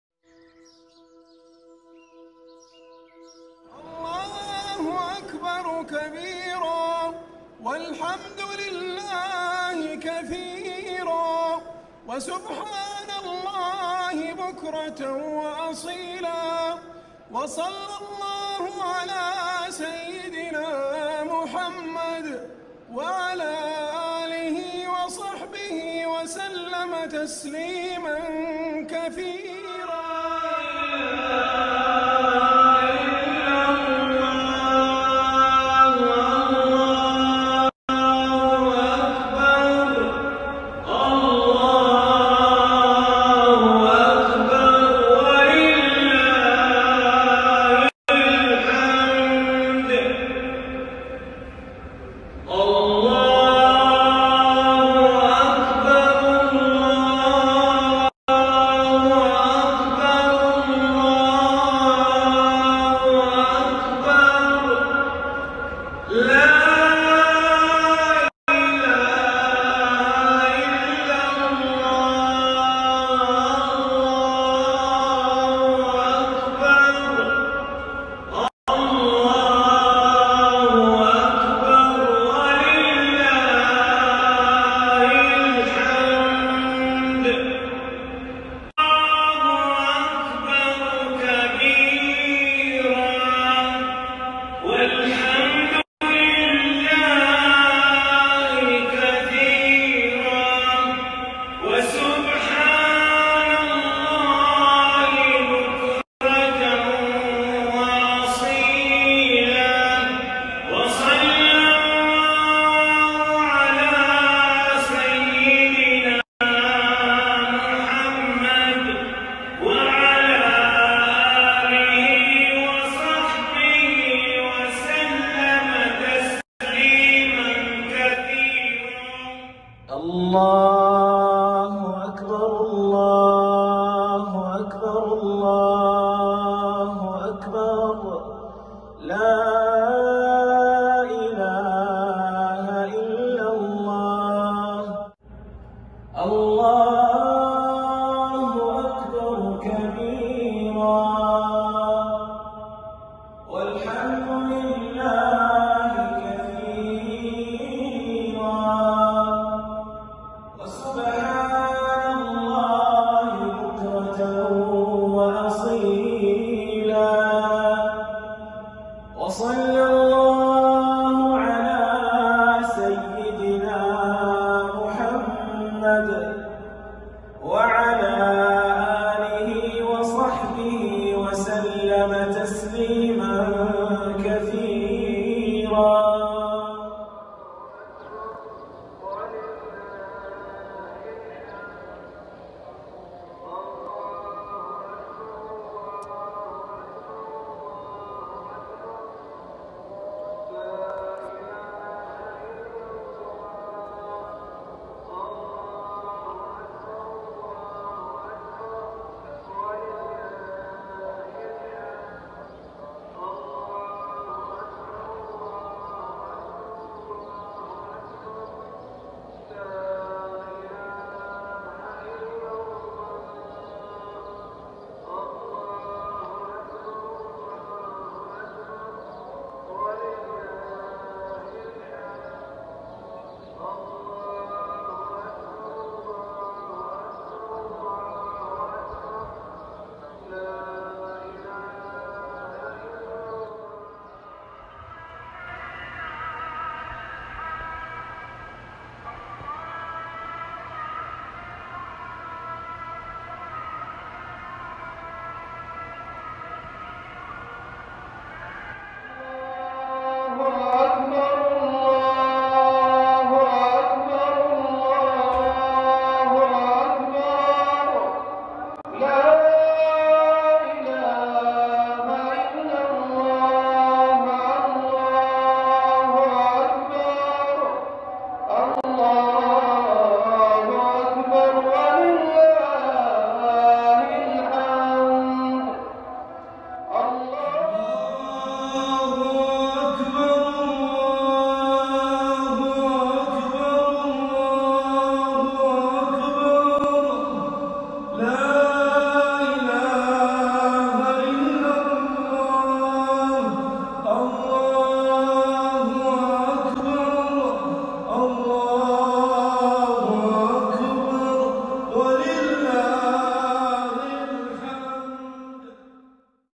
Suasana Lebaran Dengan Takbiran di Makkah,Madinah,Jeddah - Saudi Arabia.
beginilah suasana hari raya pagi hari di Saudia Arabia,Makkah,Madinah,Jeddah,sekalipun tidak bisa sholat Idul Fitri di Mesjid  namun penduduk Arab Saudia merayakan di rumah masing-masing, sesudah subuh suara takbir terus menggema dari mesjid-mesjid